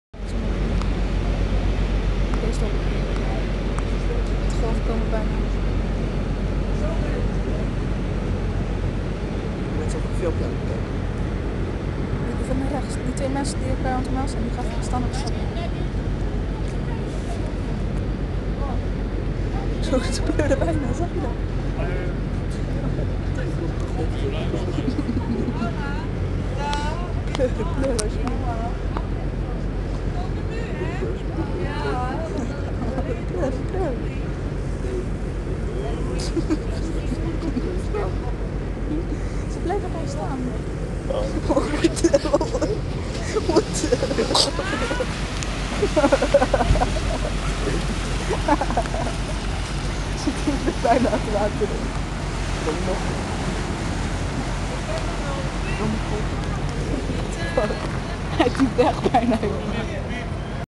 Nooit gaan varen als je dronken bent en motorpech hebt
Was wel te voorspellen